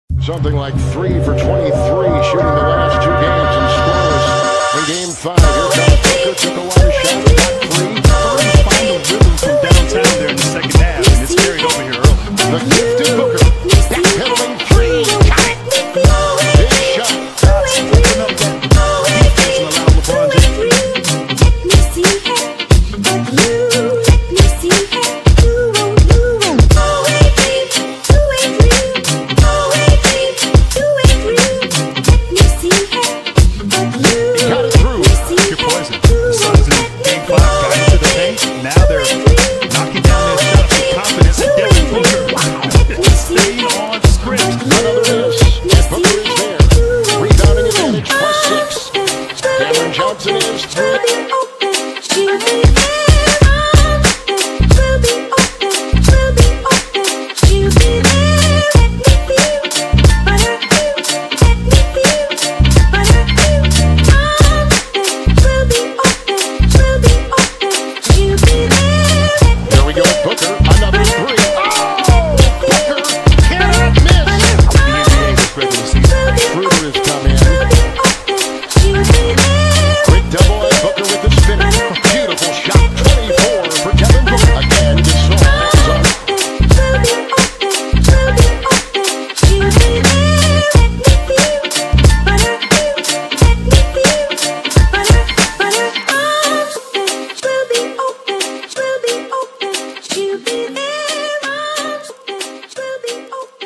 LAKERS VS SUNS - Game 6 - GOODBYE LAKERS - SUNS DEFEATED LAKERS (series 4-2)
LAKERS OUT SUNS IN